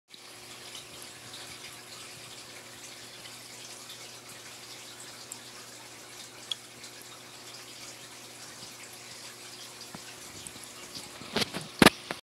Dab eating right off of my hand.
Veiled chameleon